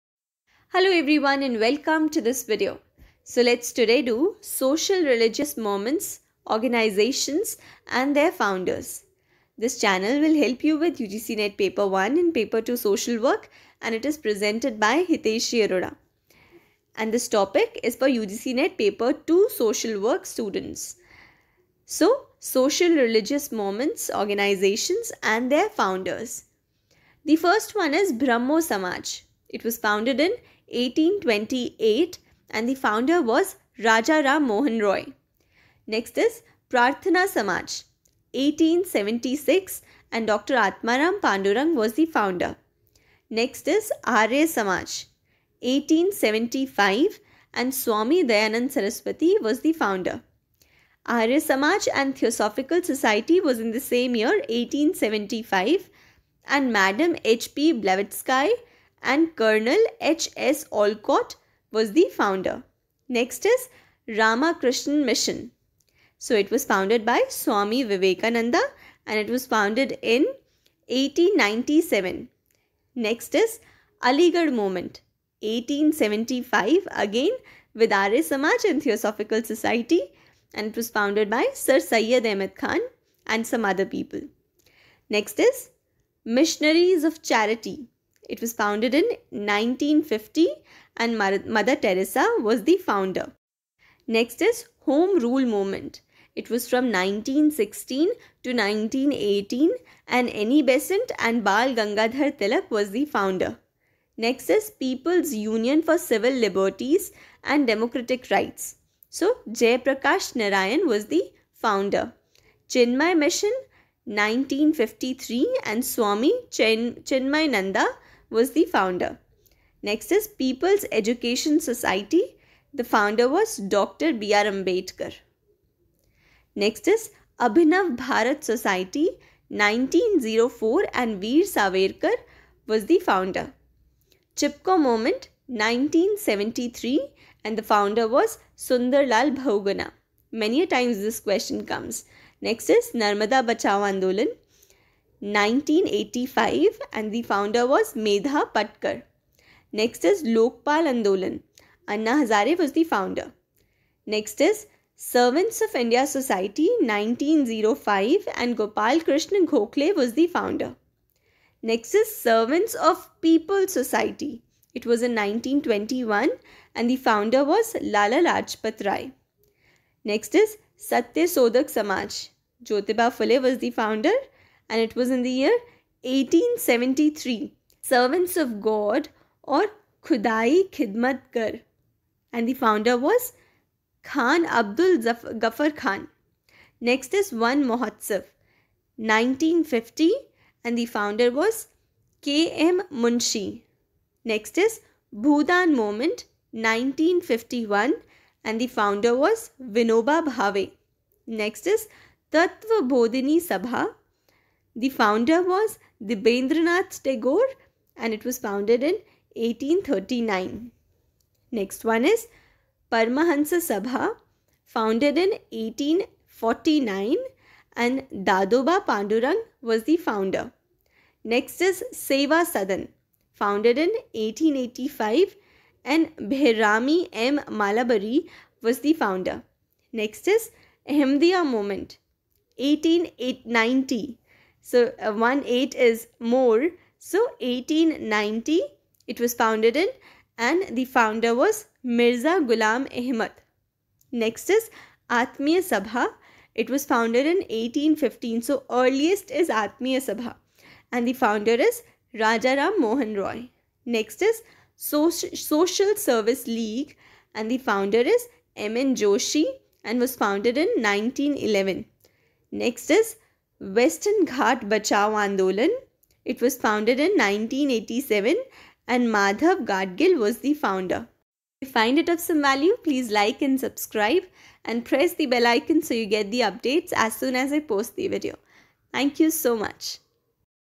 Socio Religious Movements/Organizations and their Founders ||UGCNET PAPER2|| Social Work
UGC NET PAPER 1 AND PAPER 2 (Social Work)
PRESENTED BY HITESHI ARORA

Join the telegram channel (SOCIAL WORKER'S WORLD) you will get  Quiz on previous year questions related to UGC NET PAPER 2 (Social Work) exam